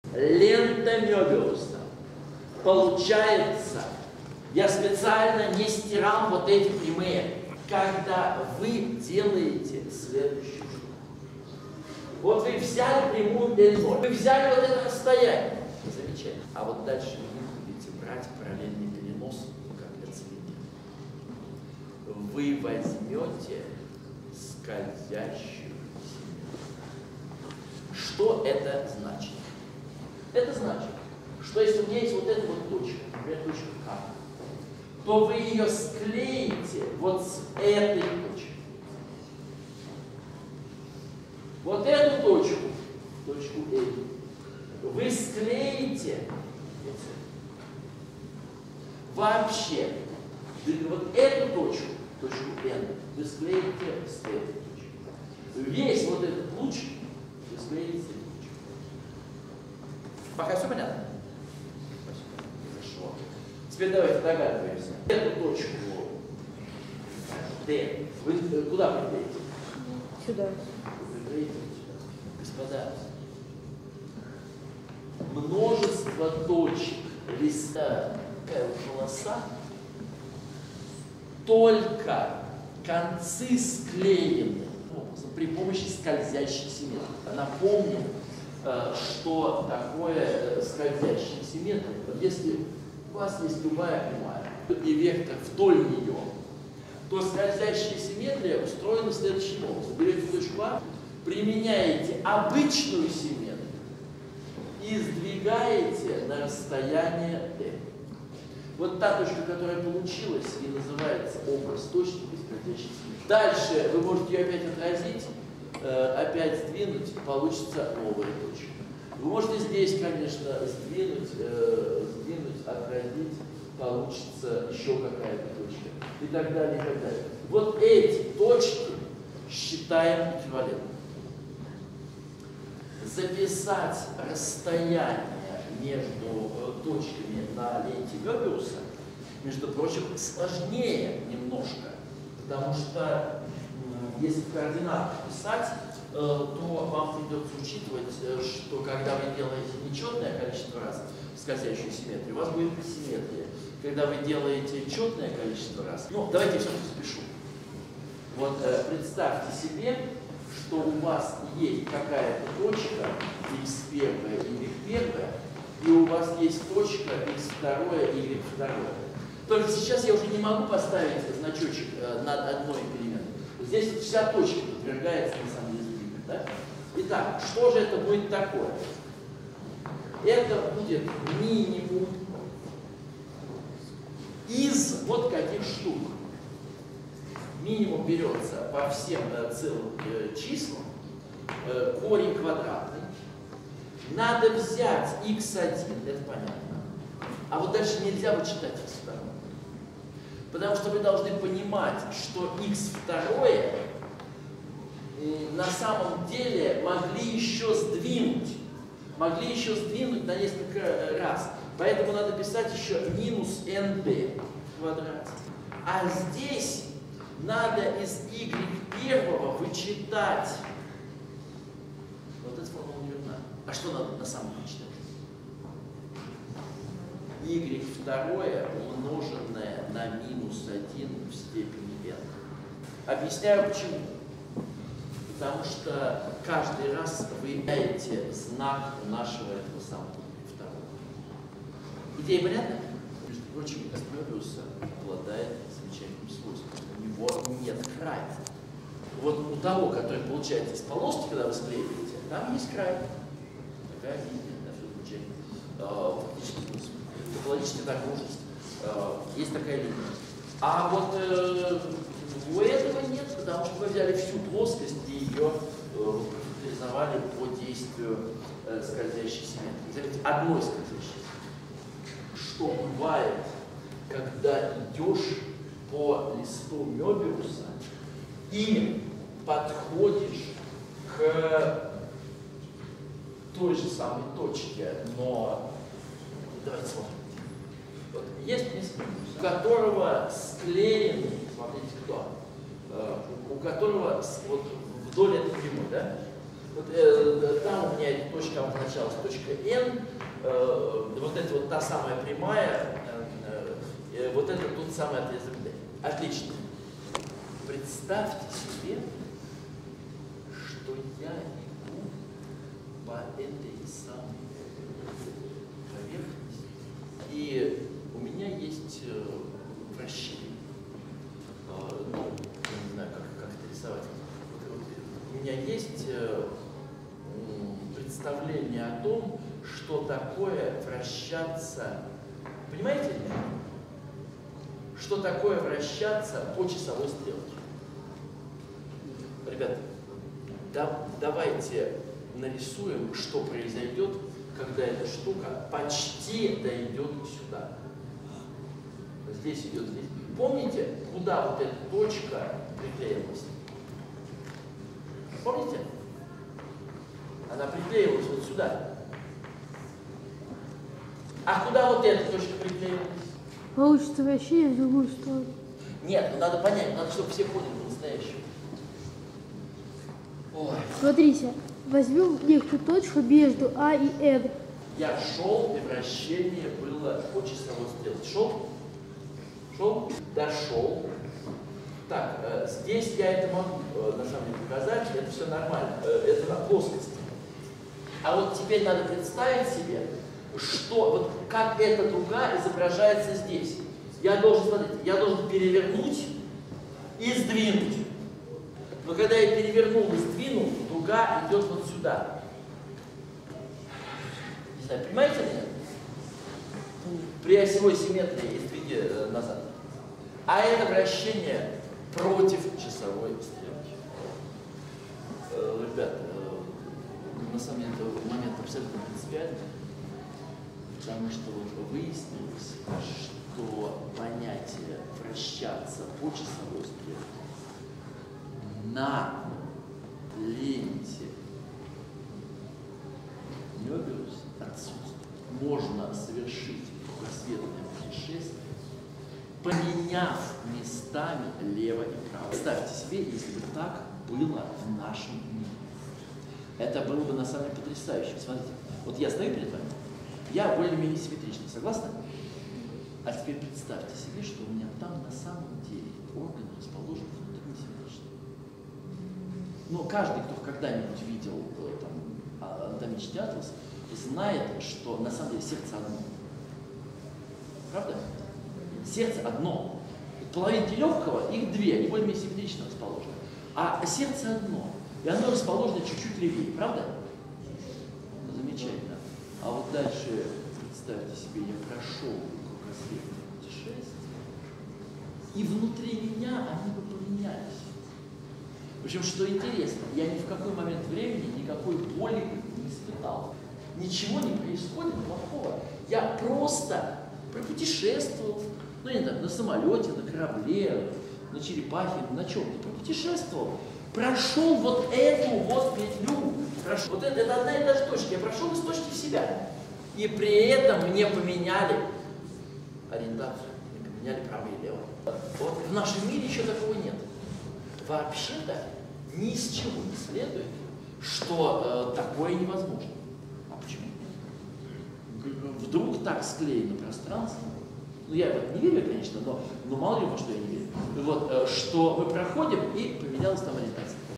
97 Лист Мёбиуса и скользящая симметрия
Из полосы можно сделать ленту Мёбиуса,  из всей плоскости — лист Мёбиуса.